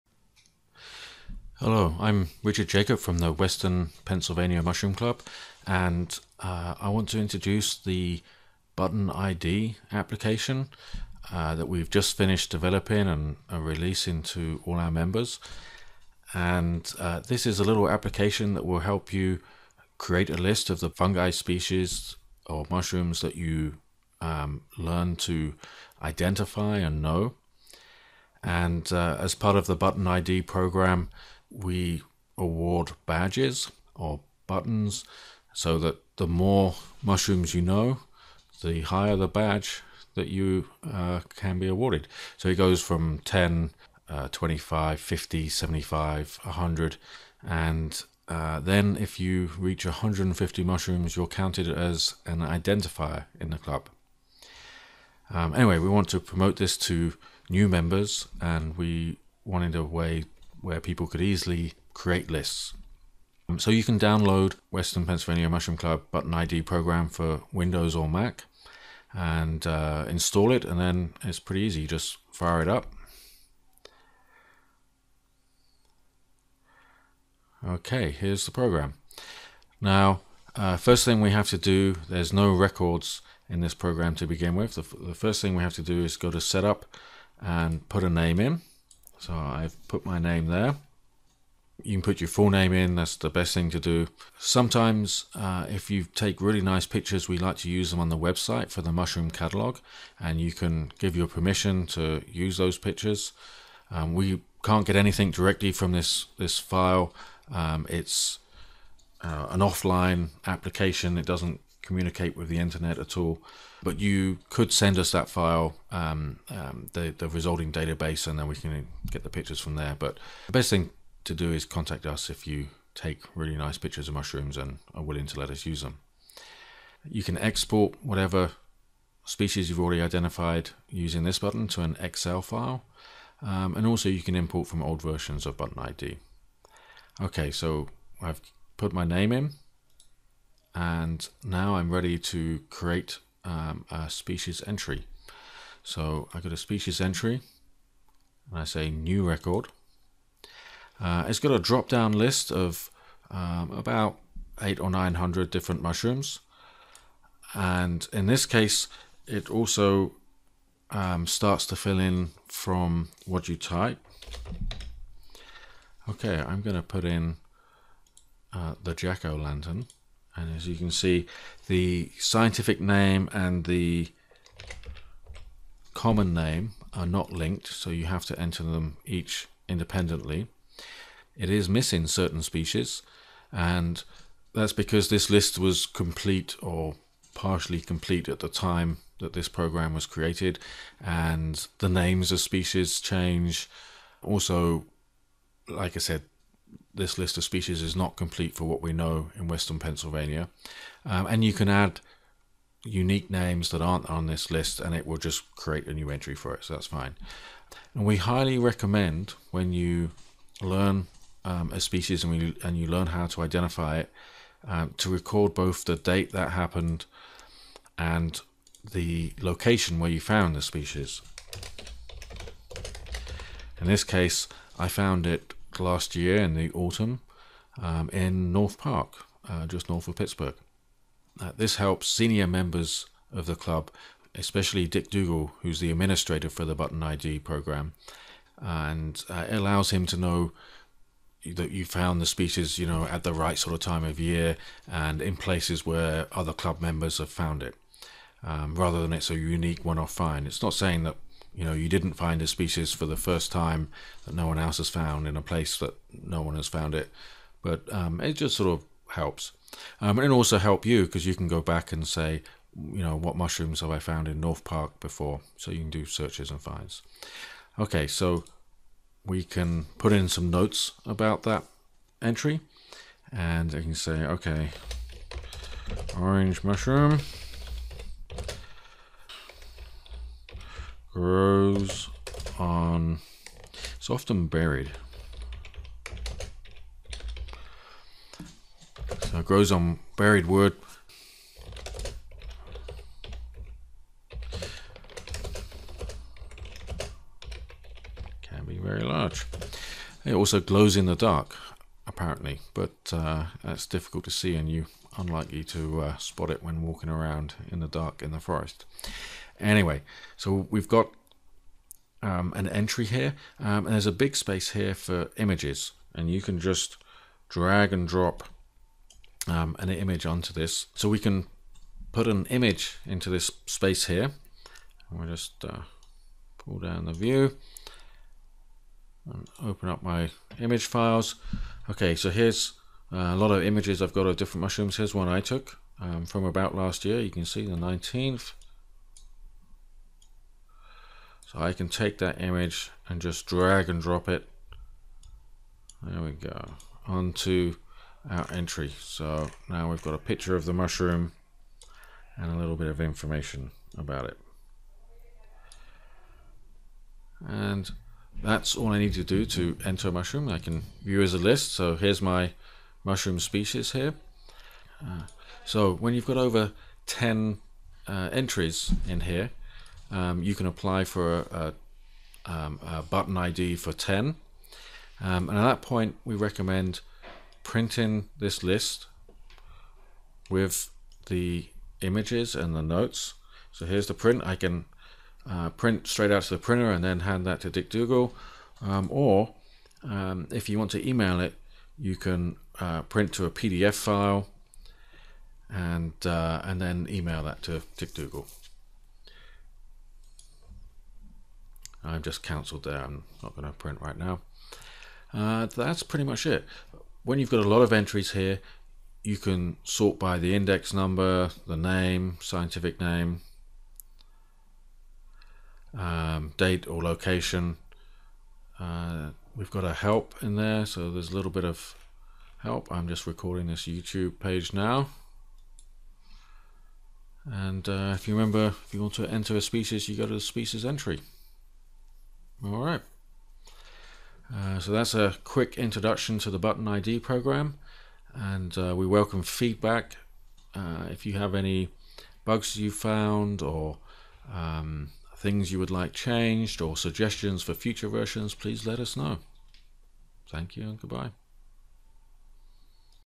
A quick guide to the Button ID application
Introducing the Button ID program and an application that runs on Windows and Macs that helps you keep track of the species that you know.